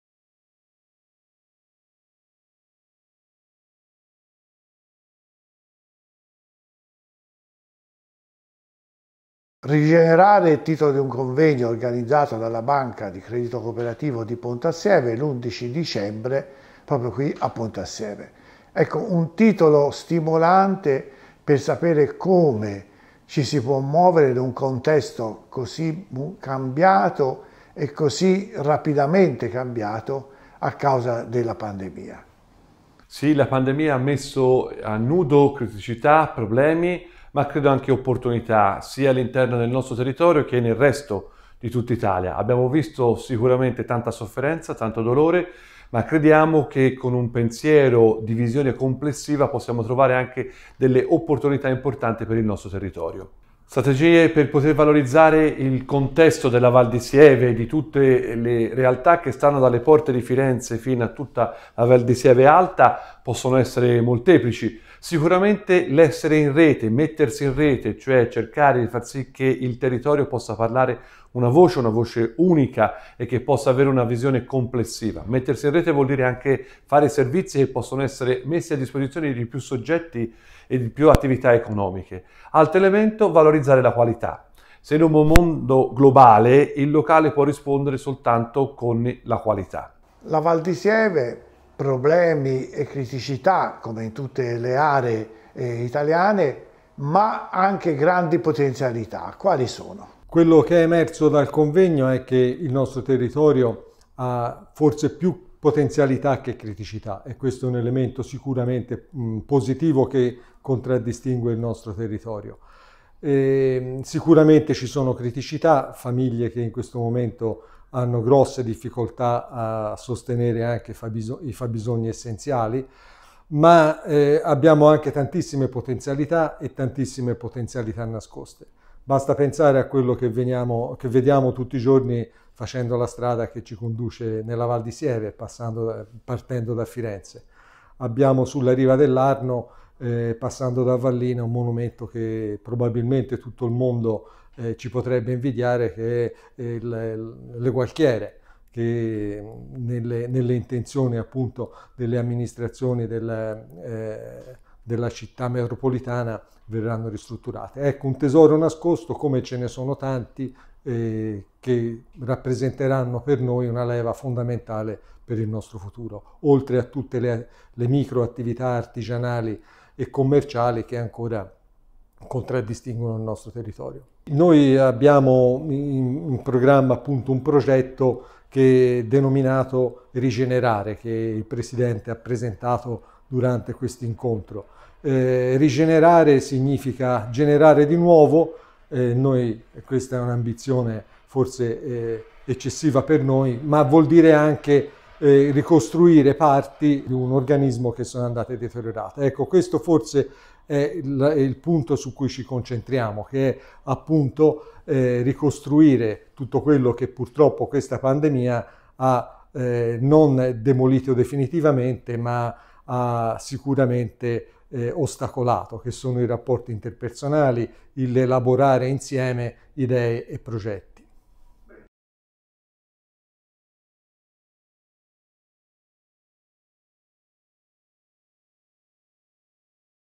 Progetto Ri-generare - In avvio i "Laboratori di idee" - Stamptoscana
La pandemia ha messo tutti a dura prova ma ha anche posto tanti interrogativi sulle nostre debolezze. Tante sono le preoccupazioni su come ripartire, soprattutto come ripensarsi in un contesto che è, senza dubbio, profondamente e rapidamente cambiato.

In questa ottica la BCC di Pontassieve ha deciso di dare un contributo fattivo con il progetto “Ri-generare”. La prima fase è stata rappresentata dal Convegno di cui parliamo in questo video con le interviste al Presidente della Bcc di Pontassieve Matteo Spanò e al Direttore generale Giovanni Vezzosi.

Adesso, come preannunciato nel Convegno, si stanno avviando i Laboratori di idee”.

I LABORATORI DI IDEE PER IL TERRITORIO, APERTI A TUTTA LA COMUNITA'
18 marzo, ore 20.45 - PNRR e PSR. Risorse per lo sviluppo dell’economia
Villa Poggio Reale, Rufina -Interviene: Stefania Saccardi – Vice Presidente Regione Toscana
31 marzo Ri-generazione territoriale
Un’alleanza per il territorio: turismo, agricoltura, sostenibilità
12 aprile Ri-generazione per lo sviluppo
Un’alleanza per la crescita: le imprese e le transizioni (ecologica, digitale, generazionale, del lavoro, socio-demografica, di genere)
5 maggio Ri-generazione sociale
Alleanze di prossimità: piccolo commercio, reti sociali e sfide digitali
19 maggio Ri-generazione della comunità
Un’alleanza tra la società civile: i beni comuni, le reti locali e welfare
9 giugno Ri-generazione della persona
Alleanze per il presente e per il futuro: cultura, formazione, educazione.

I laboratori si svolgeranno in Sala Don Maestrini a Pontassieve dalle 20.45 alle 23.00. Nel caso in cui venisse superata la capienza massima prevista dalle norme Covid, sarà comunicato ai prenotati il nuovo luogo degli incontri.